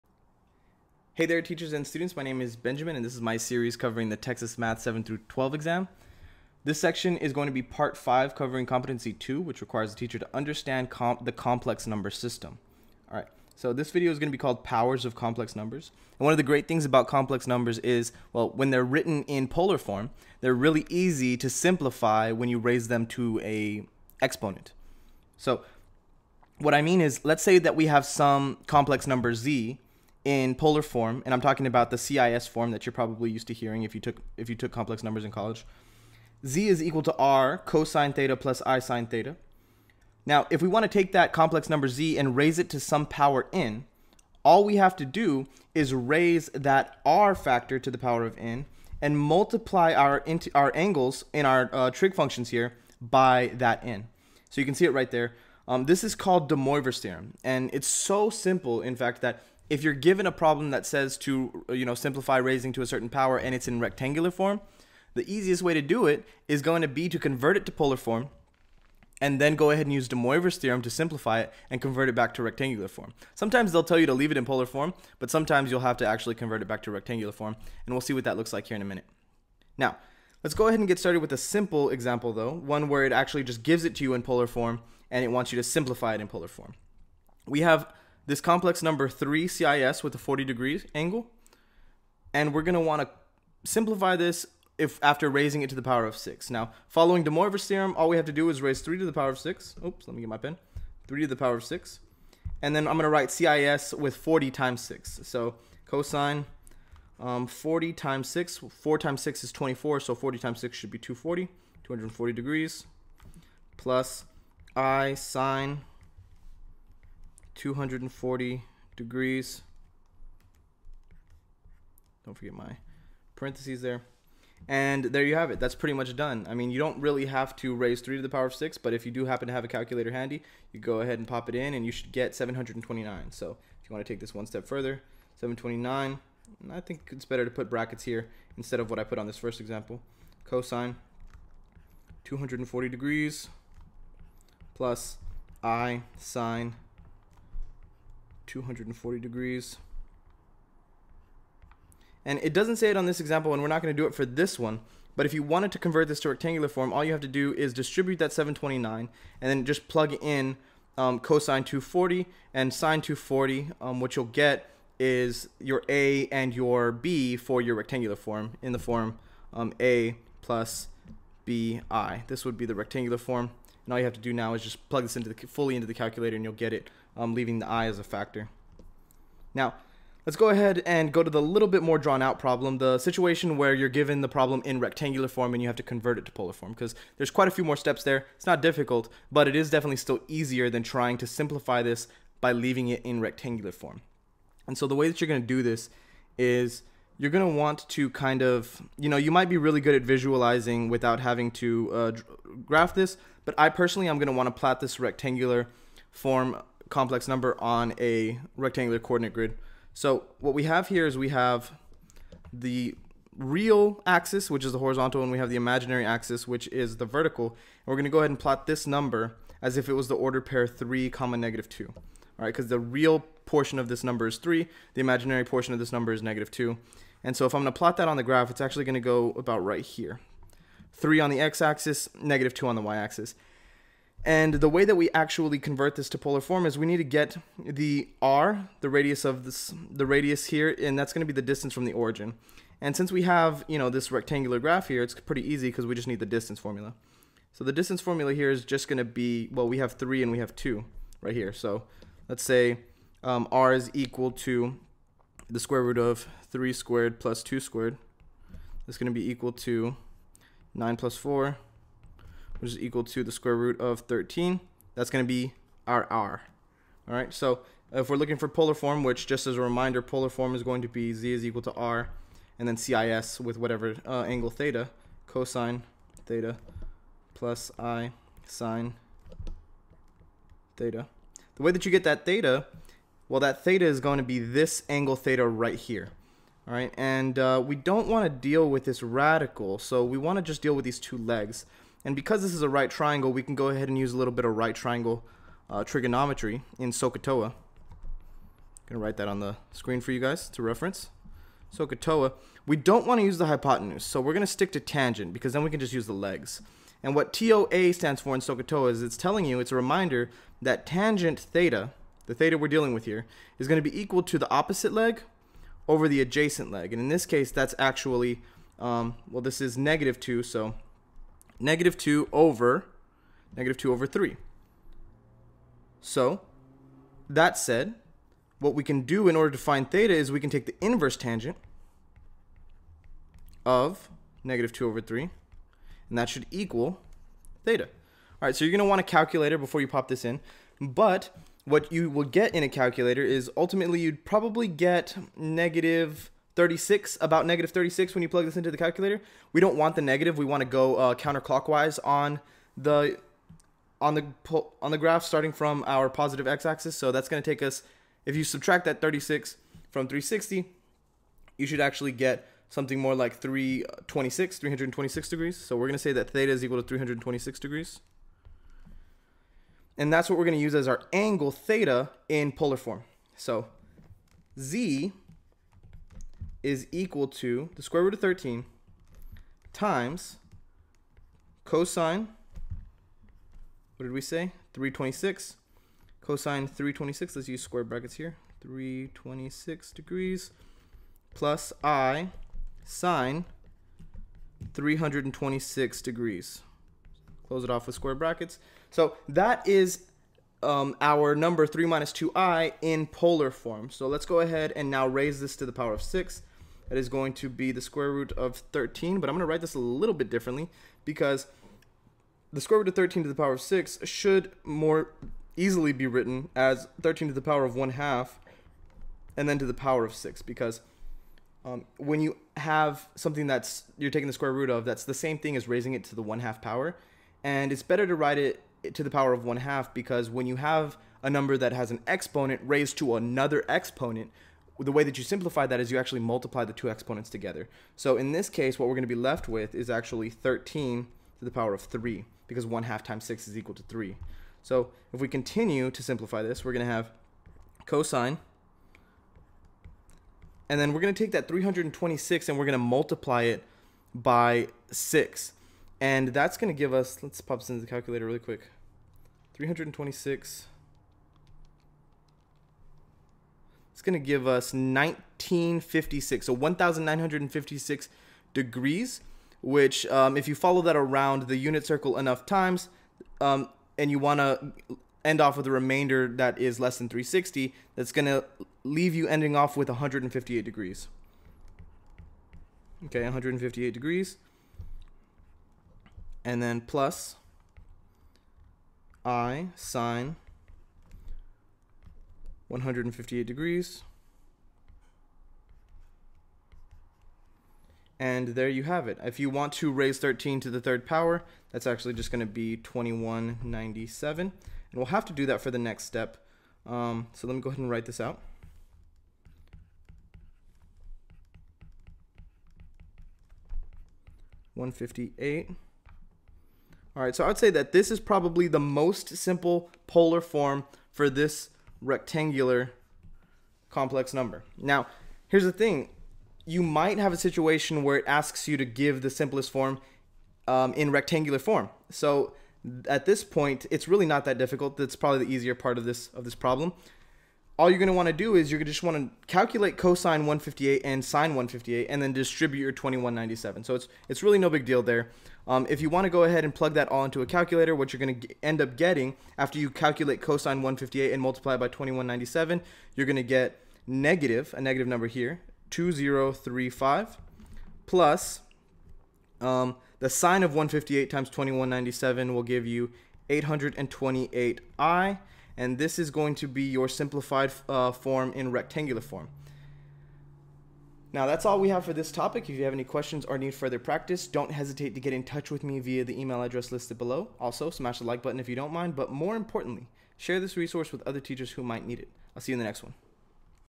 TExES Math 7-12 Competency-002 Part 5 - Powers of Complex Numbers
The competency statements for Competency 002 for the exam are listed below:

Competency 002—The teacher understands the complex number system and its structure, operations, algorithms and representations.

This Competency assumes the beginning teacher:

    A. Demonstrates how some problems that have no solution in the real number system have solutions in the complex number system.

    B. Understands the properties of complex numbers (e.g., complex conjugate, magnitude/modulus, multiplicative inverse).

    C. Understands the algebraic structure of the complex number system and its subsets (e.g., complex numbers as a field, complex addition as vector addition).

    D. Selects and uses appropriate representations of complex numbers (e.g., vector, ordered pair, polar, exponential) for particular situations.

    E. Describes complex number operations (e.g., addition, multiplication, roots) using symbolic and geometric representations.